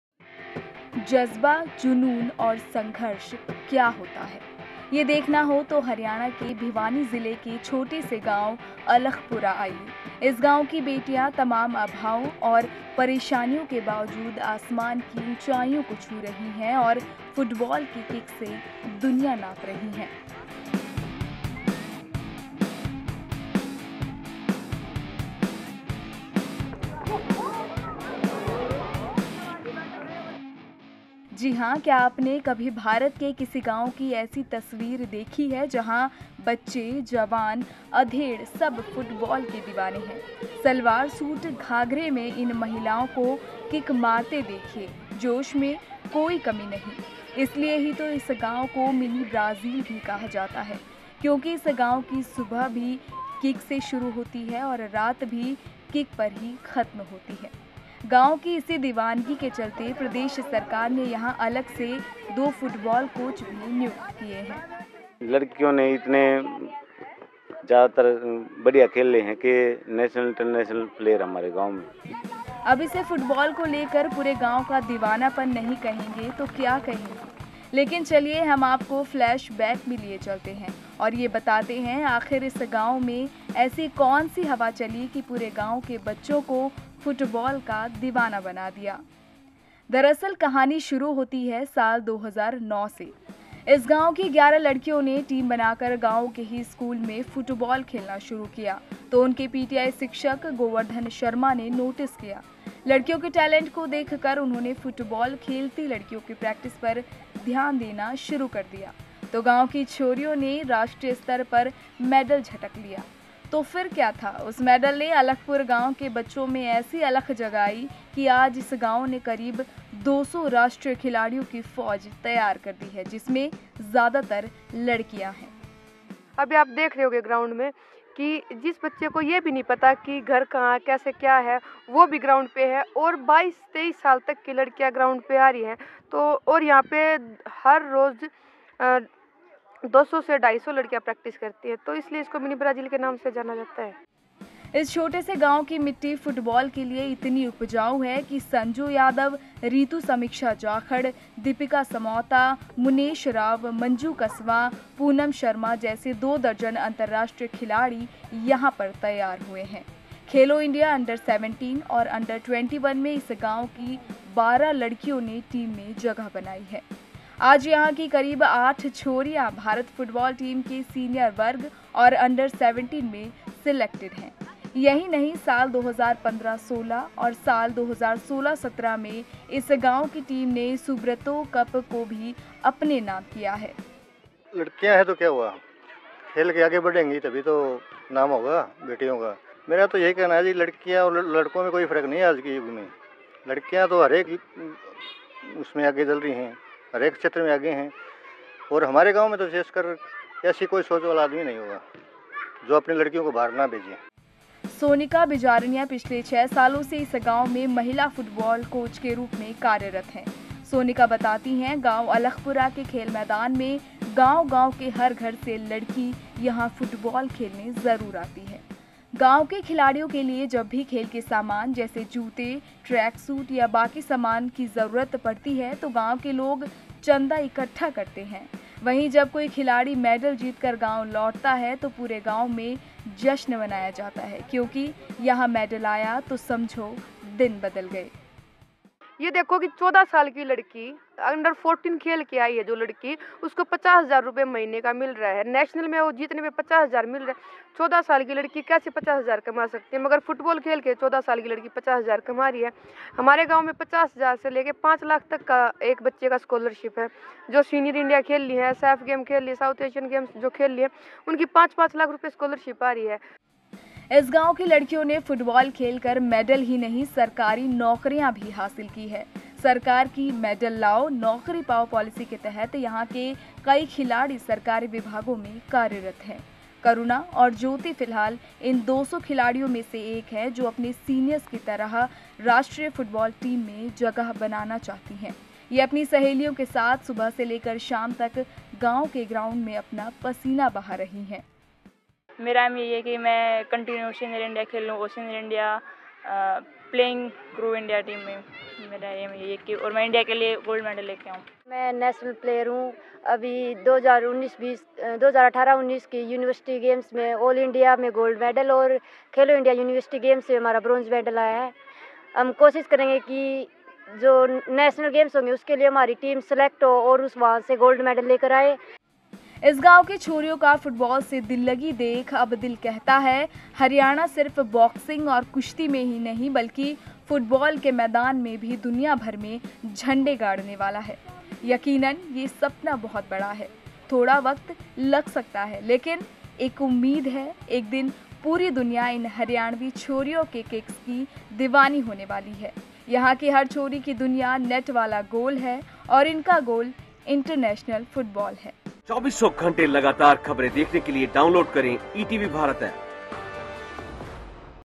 हरियाणा का ये गांव है देश का 'मिनी ब्राजील' | Mini Brazil in Haryana | ETV Bharat Uttarakhand
जज्‍बा, जुनून और संघर्ष क्‍या होता है, यह देखना हो तो हरियाणा के भिवानी जिले के छोटे से गांव अलखपुरा आ जाइए. इस गांव की बेटियां तमाम अभावों और परेशानियों के बावजूद आसमान की ऊंचाइयों को छू रही हैं,और फुटबॉल के किक से दुनिया नाप रही हैं.